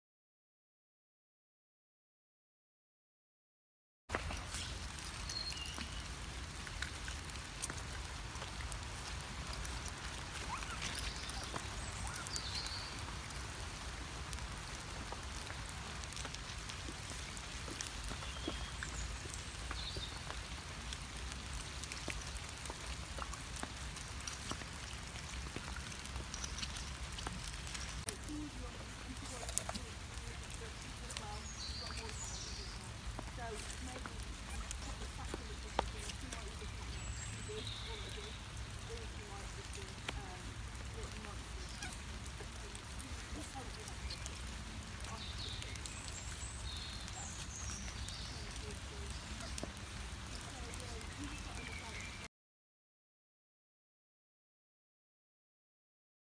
Otter treat
DZG's otters enjoyed a tasty treat when a crab was discovered in the regular fish delivery.